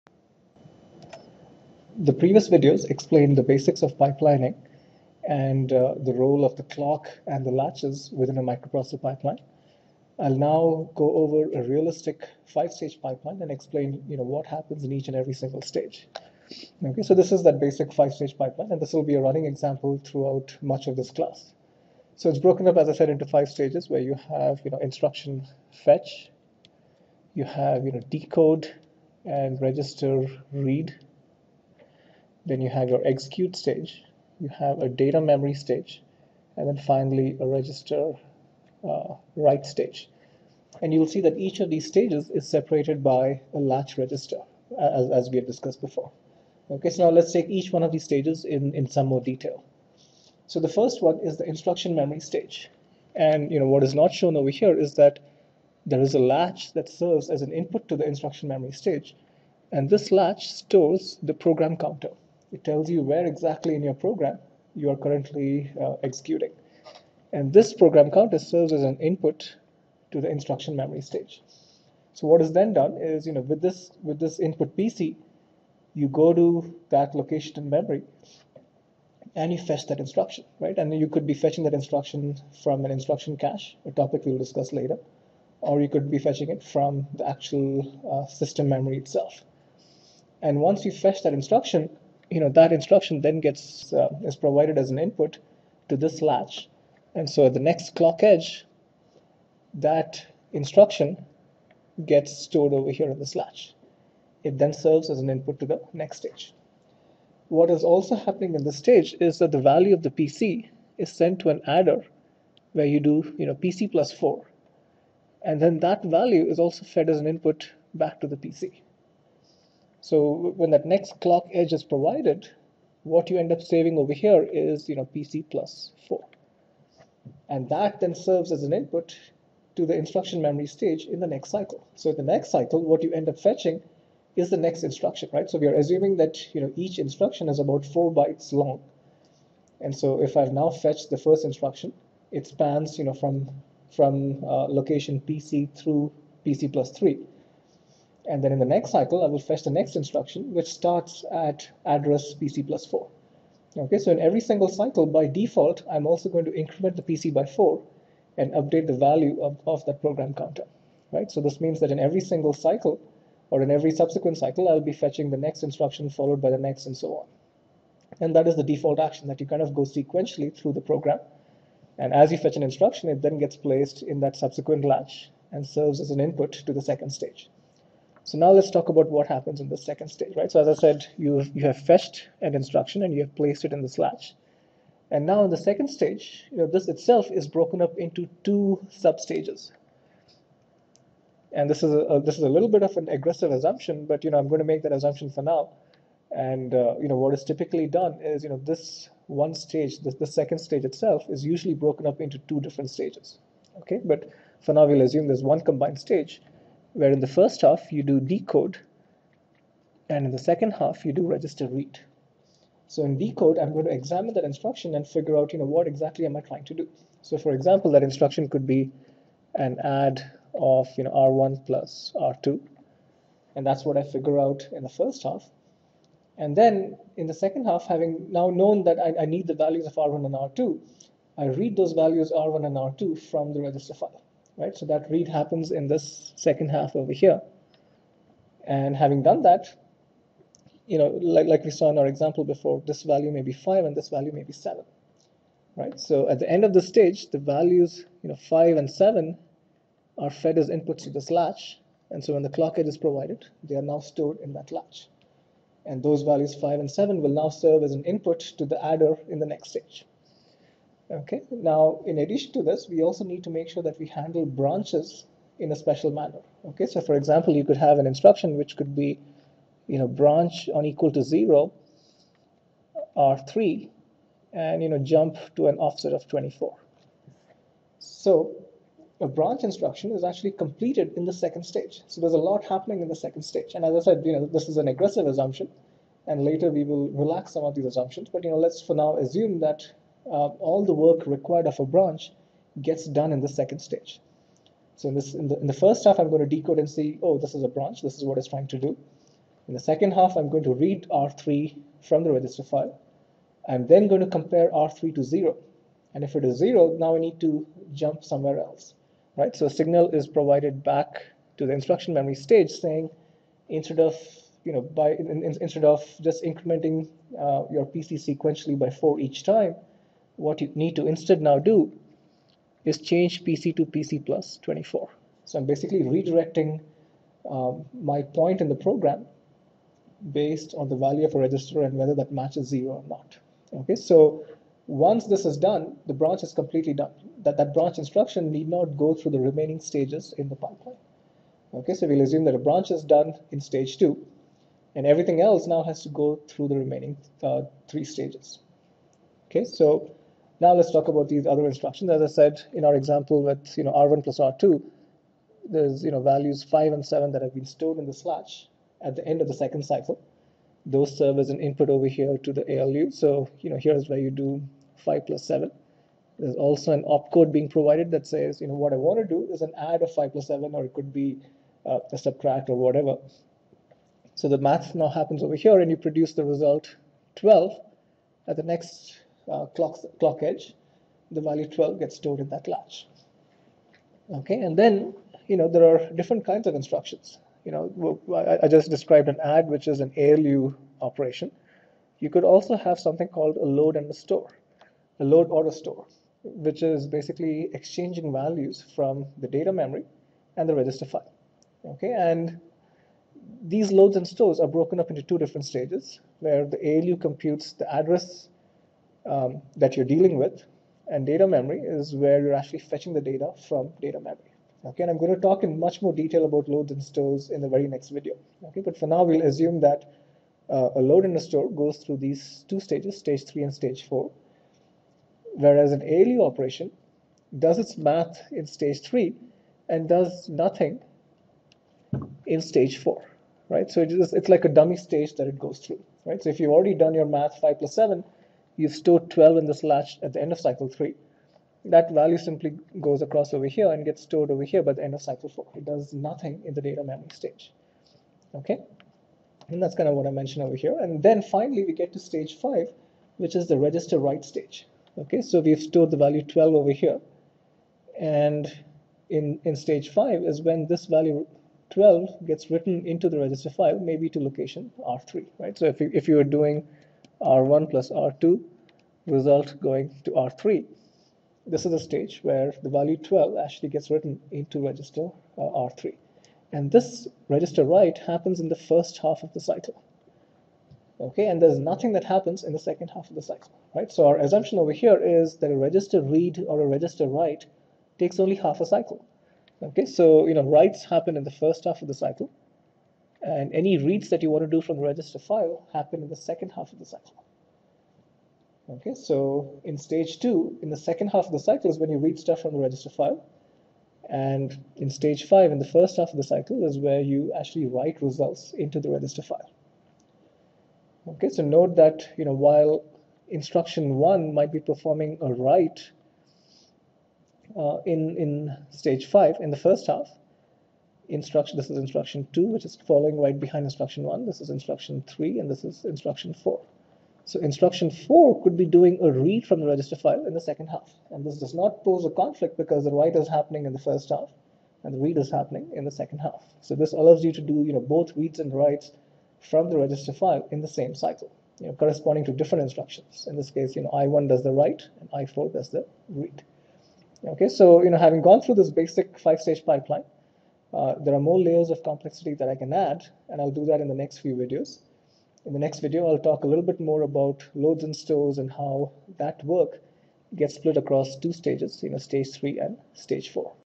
CS6810 -- Lecture 6. Computer Architecture Lectures on Pipelining
CS6810 Computer Architecture, University of Utah.
Instructor: Prof. Rajeev Balasubramonian.
Course for senior undergraduates or early-stage graduate students.
Topic: Pipelining (Lecture 3 of 11).  Description of the basic 5-stage pipeline.